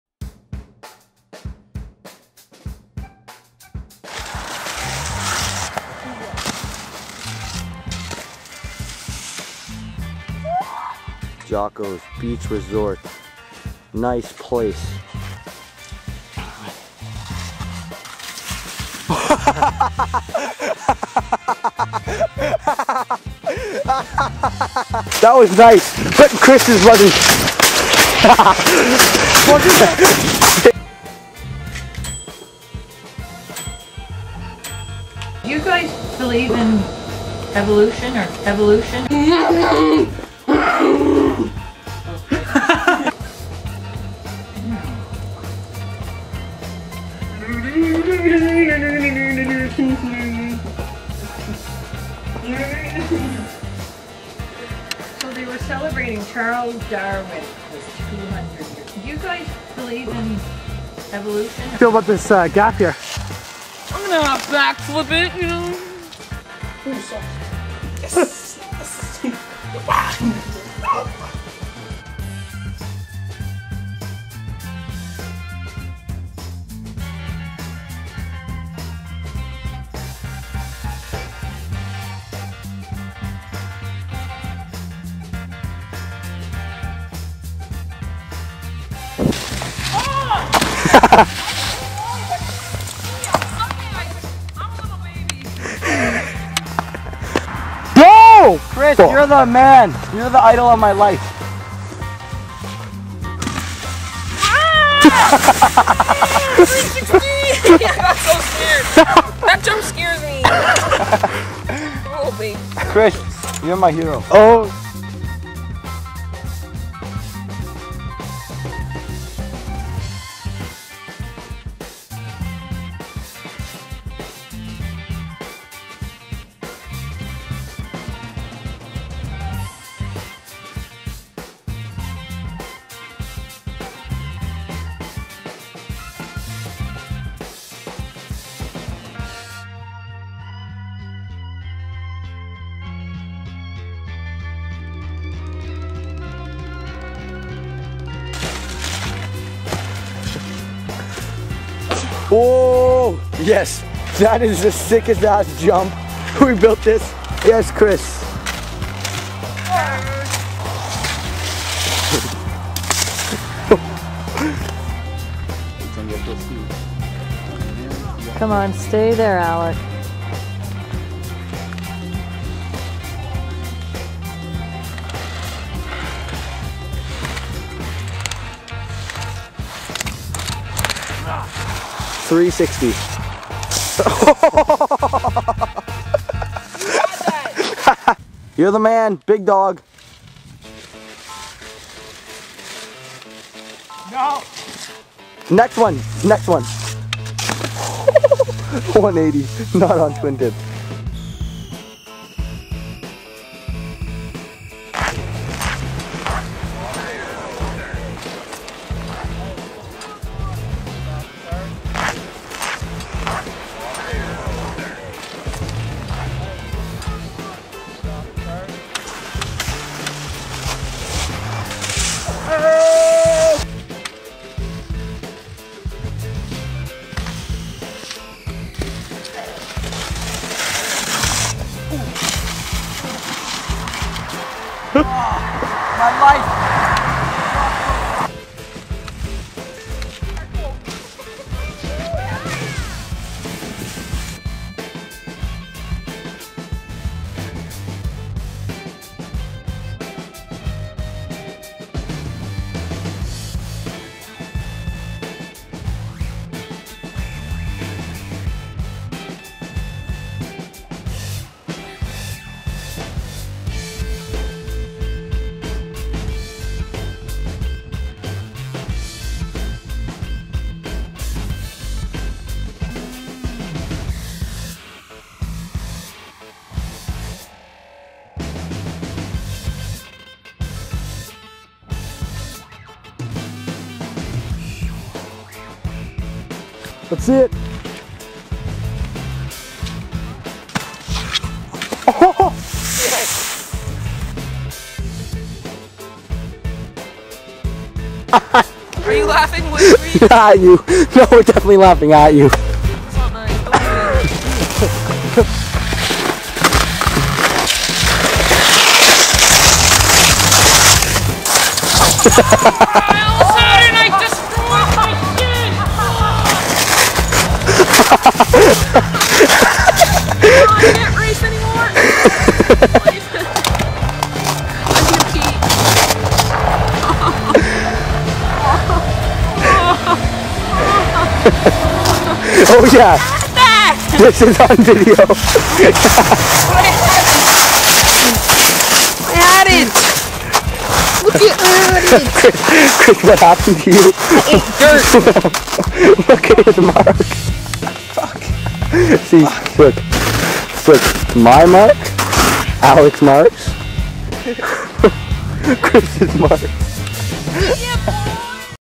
speed camp
chris and alec and i chillin at calabogie and jocko's motel (sweet place)